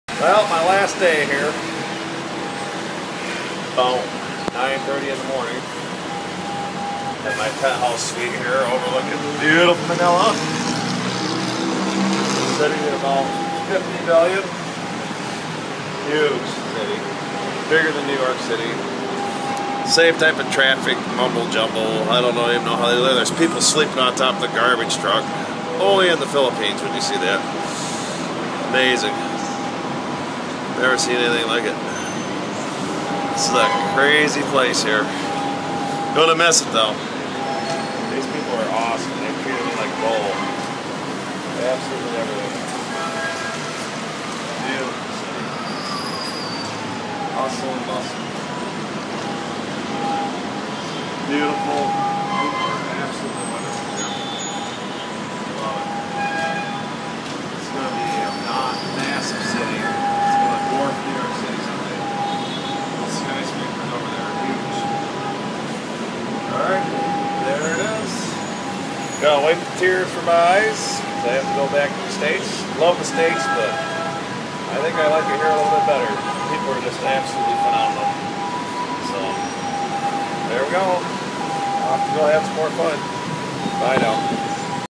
Phillippine 2013 vacation Manila Hamilton in 9th floor
Staying at Hamilton penthouse sweet 9th floor very nice place